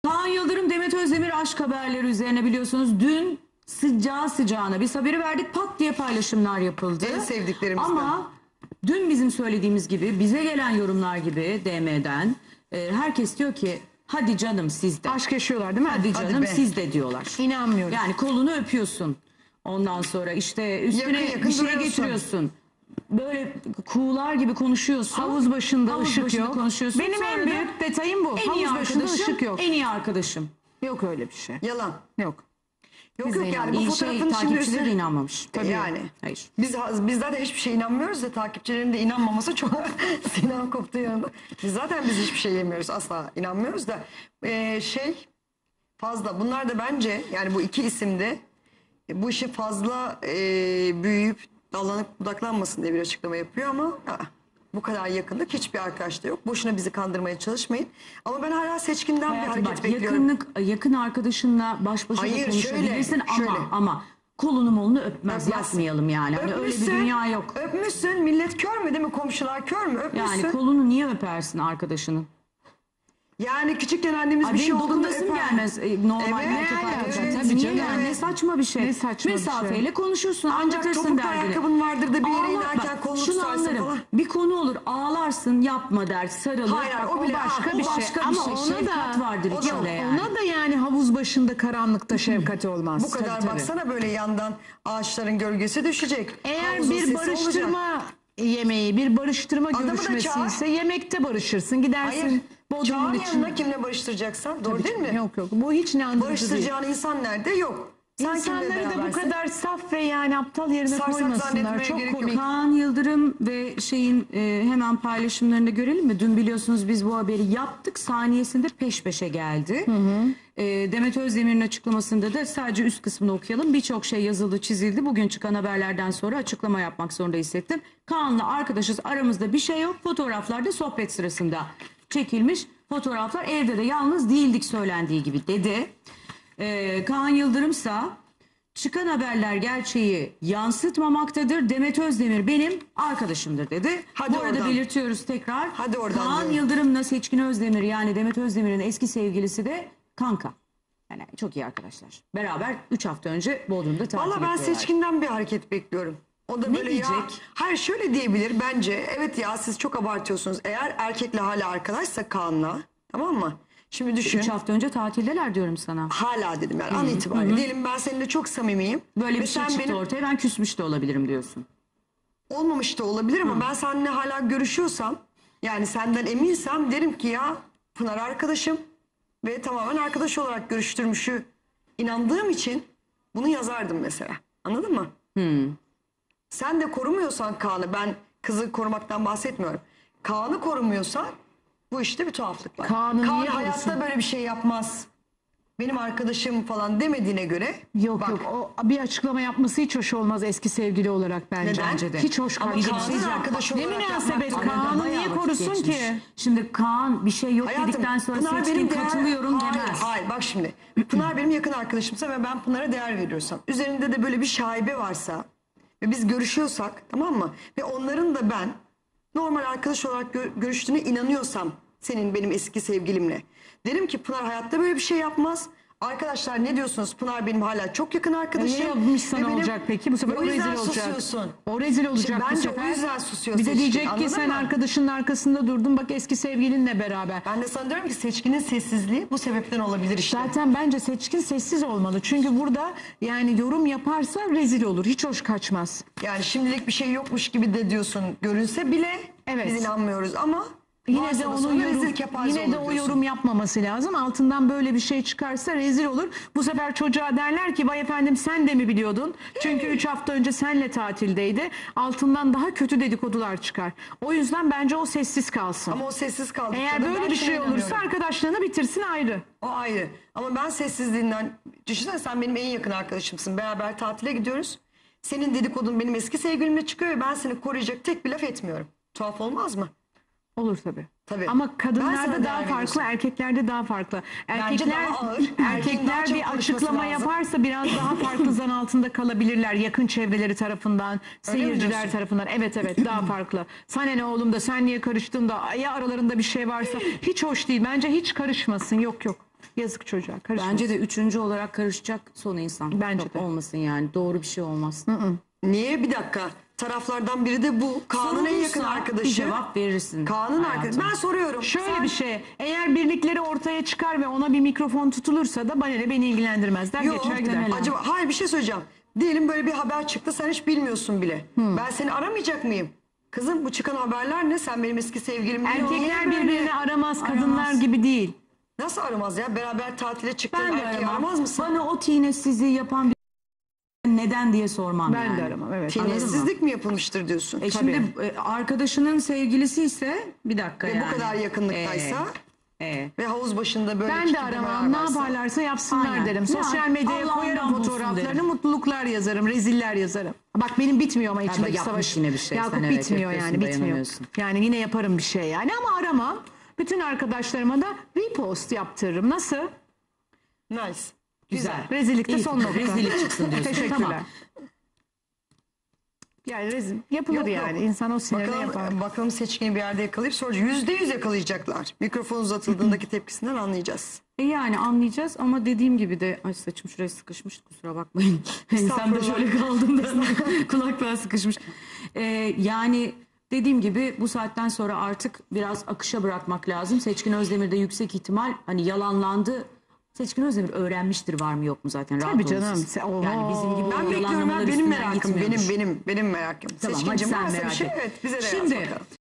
Demet Özdemir'e takipçileri neden inanmadı? Kaan Yıldırım aşkını yalanlamıştı!
Demet Özdemir'in meslektaşı Kaan Yıldırım ile yakınlaştığı iddia edilmişti.